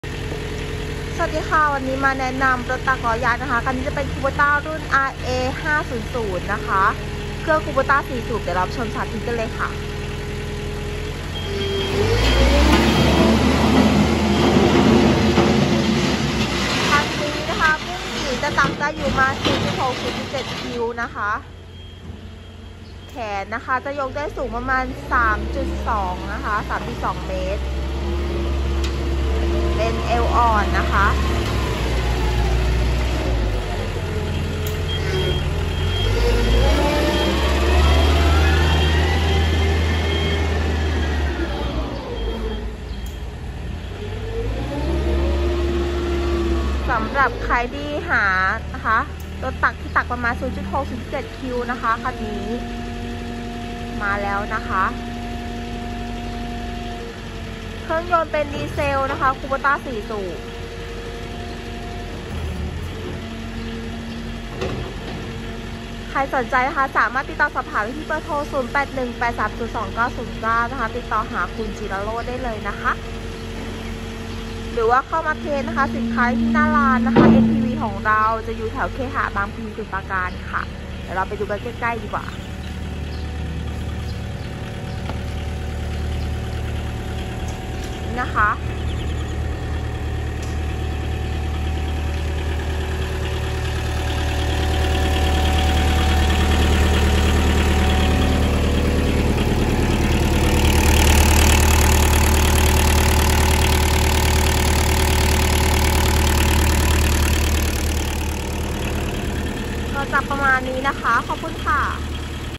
ขายรถตักล้อยาง KUBOTA รุ่น RA500 เก่านอก โทร จิรโรจน์ / Line : jiraroj2909
รถตักล้อยาง KUBOTA รุ่น RA500
เครื่องยนต์คูโบต้า 4สูบ
บุ้งกี๋ 0.6 คิว ยกสูง 3.2 เมตร
มีเก๋ง เอวอ่อน รถเก่านอก
ไม่เคยใช้งานในไทย สภาพรถพร้อมใช้งาน

MPVจักรกล จำหน่ายเครื่องจักรกลหนัก อาทิ เช่น รถขุด รถตัก รถบด
รถกระเช้า รถโฟล์คลิฟต์ รถแทร็กเตอร์ เป็นต้น